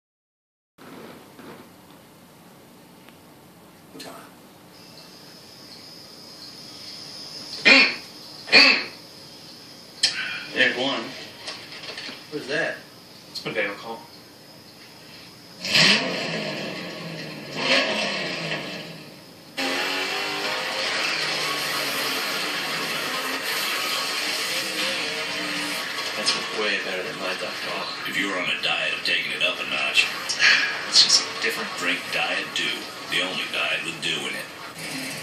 The Dale Call Commercial
Dale Earnhardt Jr new Diet MTN.Dew Commercial. The "Dale Call"